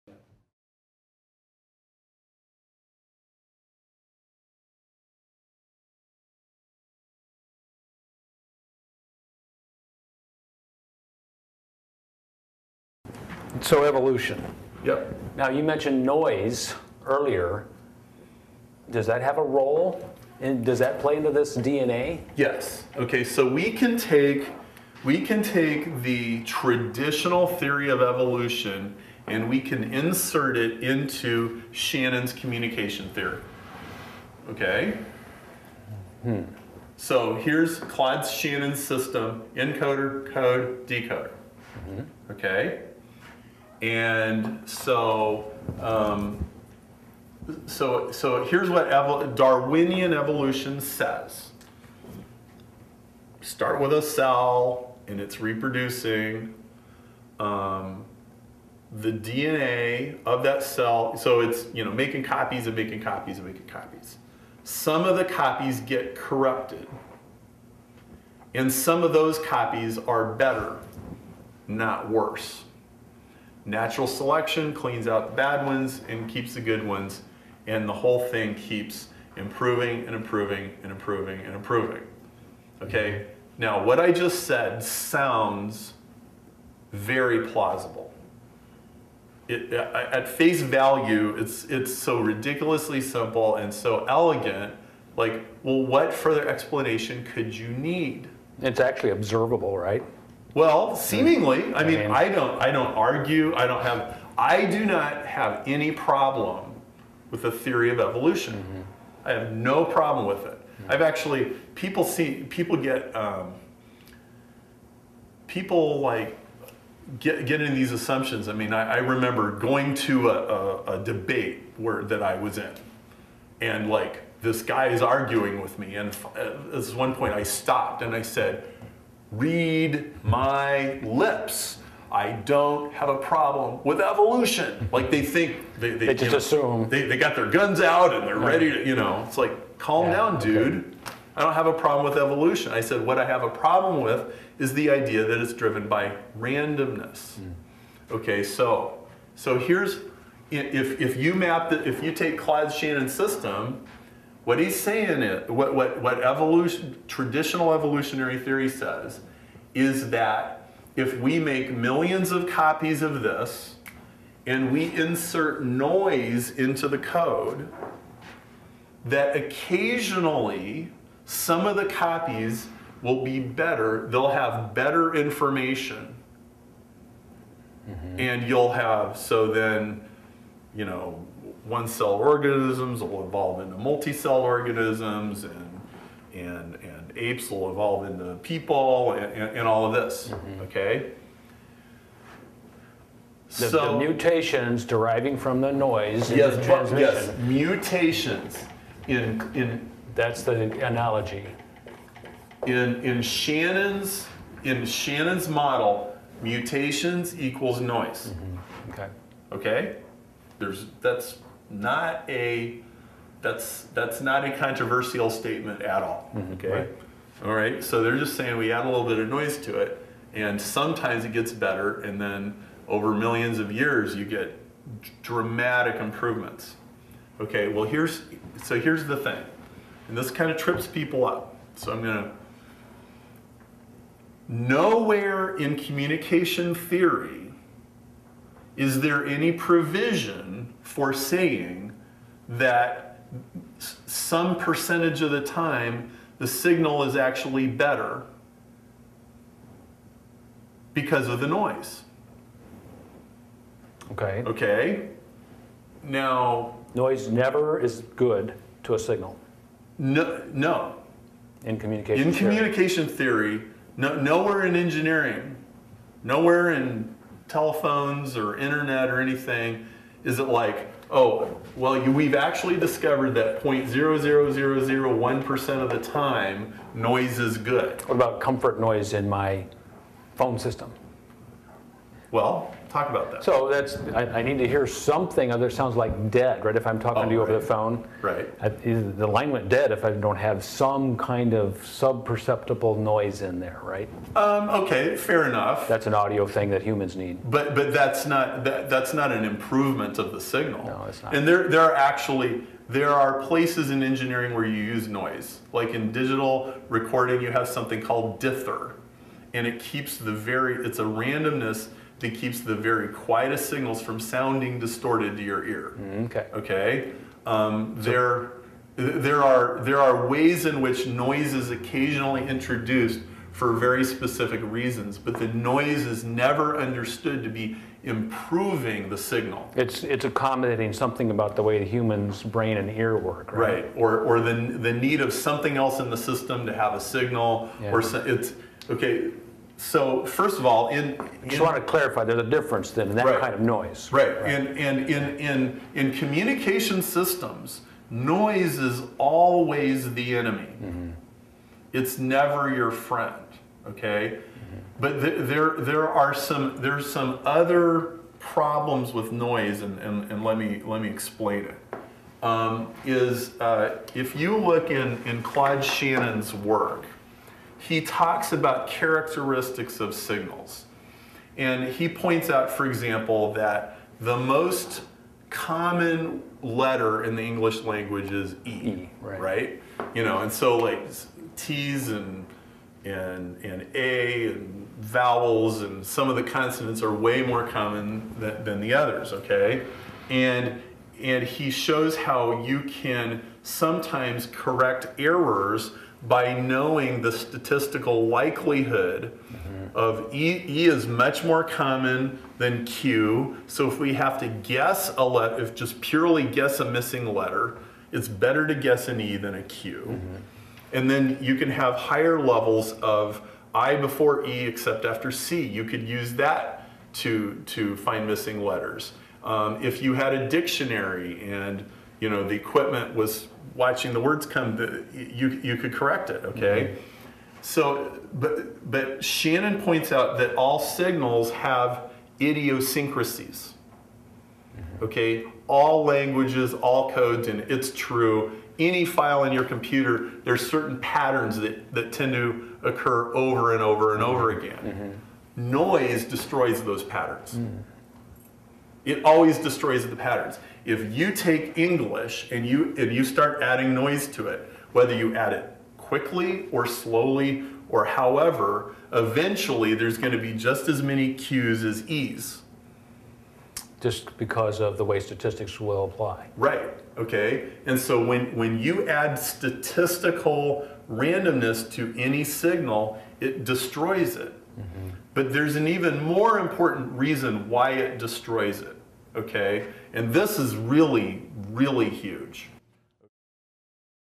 Evolution Through the Lens of Information Theory: Random Mutations and Noise
Perry Marshall, Author of "Industrial Ethernet" and Communications Engineer Bill Jenkins give a technical Treatment of Information Theory as it relates to DNA and Evolution.